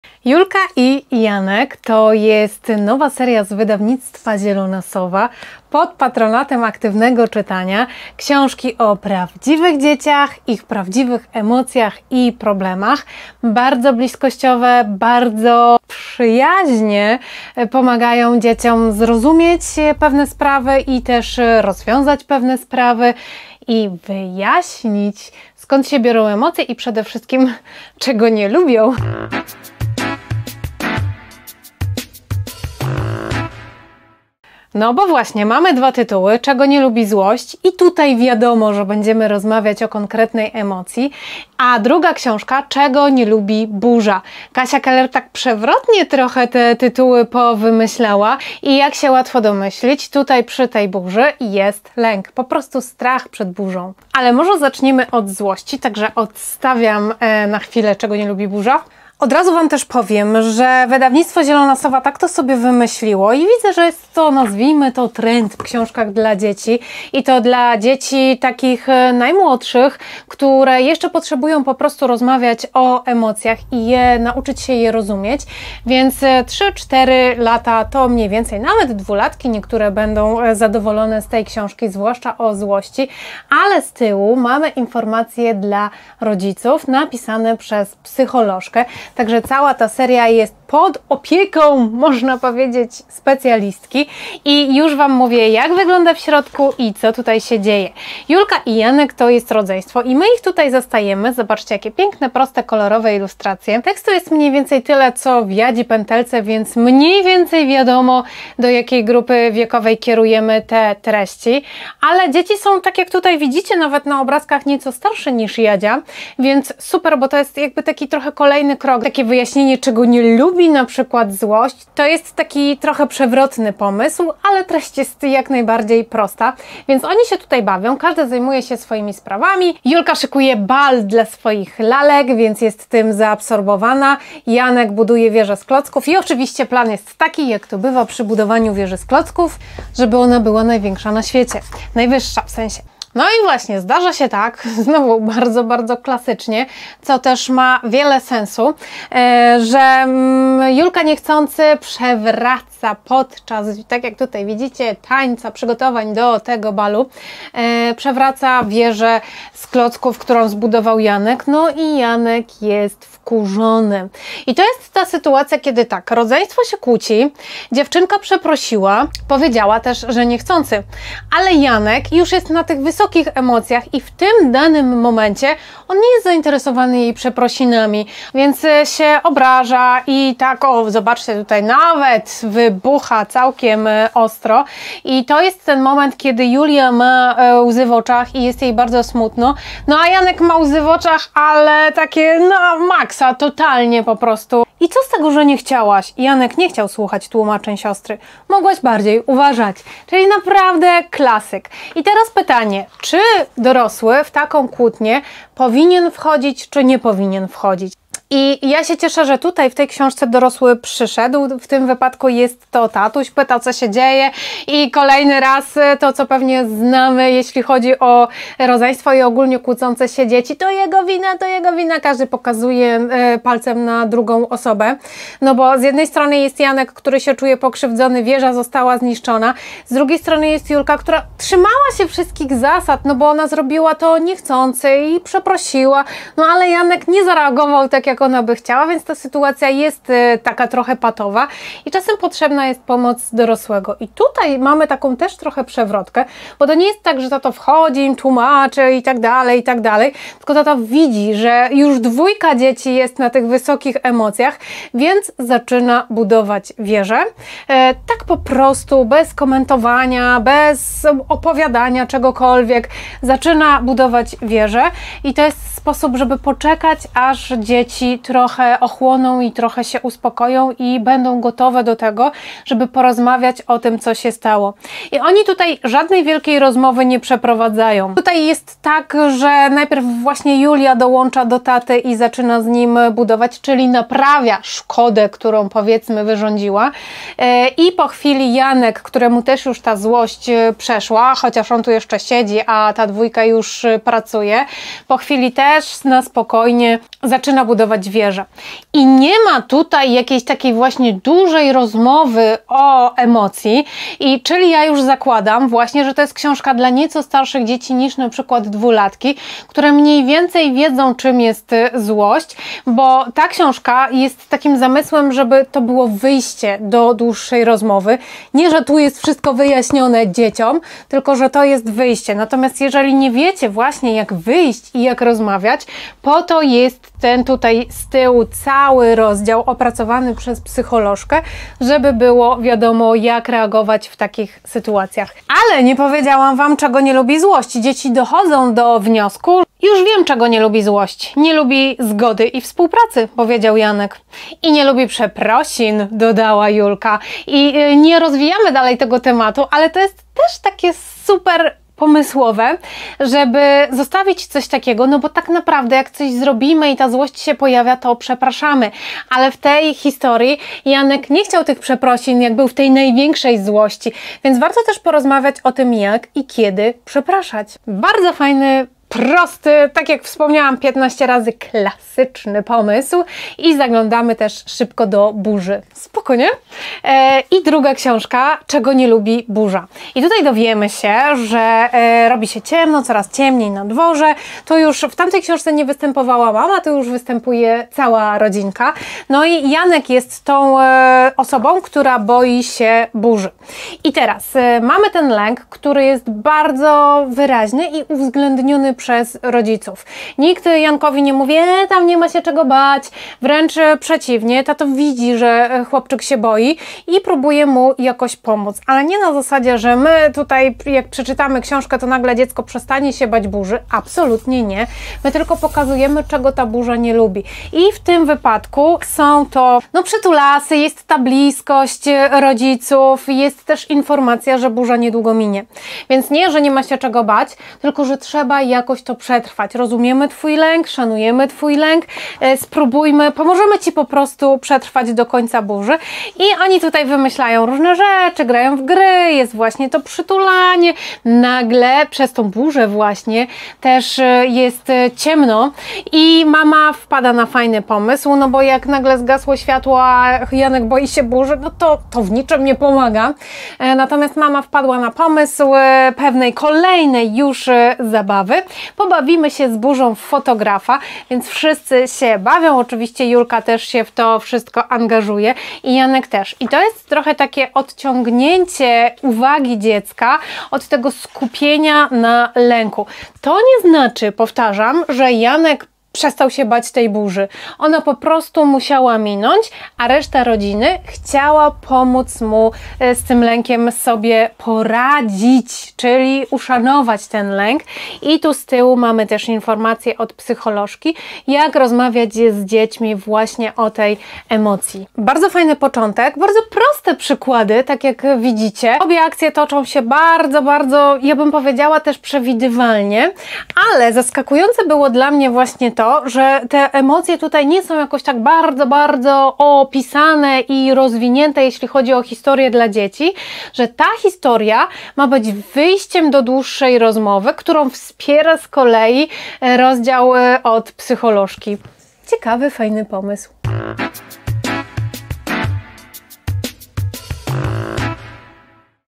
„Julka i Janek”. Czego nie lubi ZŁOŚĆ i Czego nie lubi BURZA | Recenzja KSIĄŻKI DLA DZIECI Patronat
Julka i Janek to nowa seria książek wydawnictwa Zielona Sowa. Propozycja opiekująca się poszczególnymi emocjami, na początek złość i strach.

Te książki pomagają dzieciom zrozumieć uczucia, ale też zerknąć, jak radzą sobie z nimi rówieśnicy. Autorką jest Kasia Keller, ilustracje przygotowała Karolina Krakowiecka, a uwagi dla rodziców napisała psycholożka Izabela Maliszewska.

Cała seria jest pod patronatem Aktywne Czytanie i myślę, że najlepiej sprawdzi się z dziećmi około trzeciego i czwartego roku życia.

Czego nie lubi złość

Pewnego dnia Janek układa klocki, a Julka szykuje lalki na bal. Dzieci zajęte są swoimi sprawami i tak naprawdę nie zwracają na siebie uwagi. Aż do momentu, gdy Julka kręcąc się i podśpiewując, niechcący zahacza nogą o budowlę brata, a klocki rozsypują się po całej podłodze.

Jak łatwo się domyślić, to wprawia Janka w okropną złość, której bez zastanowienia daje się od razu ponieść. Myślę, że wiele jest takich sytuacji w codzienności rodzeństwa, kiedy jedno zrobi coś niechcący, a drugie się po prostu wkurza. To zupełnie zrozumiałe, że wtedy wywiązuje się kłótnia.

Tym razem reakcją dziewczynki jest: „Przepraszam, to było niechcący” i chęć naprawienia wieży. Jednak Janek kompletnie nie ma na to ochoty – ani na przeprosiny, ani na budowanie od nowa.

Jak się zakończy ta akcja? Oczywiście, że w recenzji opowiadam ze szczegółami. O lęku przed burzą również

Na końcu każdej książki jest rozdział skierowany do rodziców pt. Zadbaj o emocje. Psycholożka podpowiada, dlaczego złość jest ważna i nie warto jej tłumić oraz jak rozmawiać z dziećmi, kiedy zdarzy się taka trudna sytuacja.

Nazywam się Anna Jankowska, jestem pedagożką.

👉 Zapisz się na Newsletter, wtedy robisz szach-mat i o wszystkim dowiadujesz się najszybciej